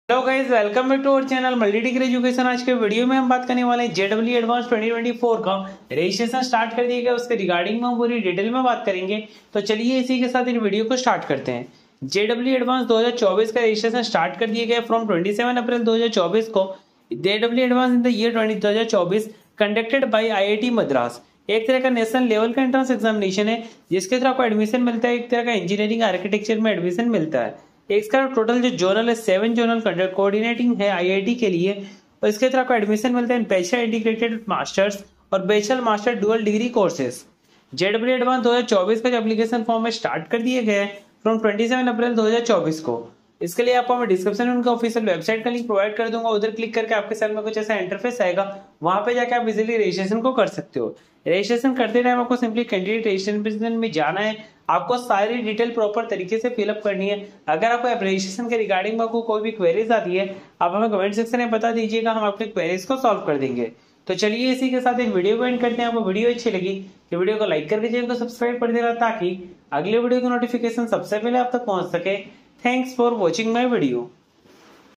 JEE Advanced 2024 Registration Started | JEE Advanced Application form Started 2024 | MDE
Is video me ham aapko JEE Advanced Registration Started 2024 ki details dene waale hai.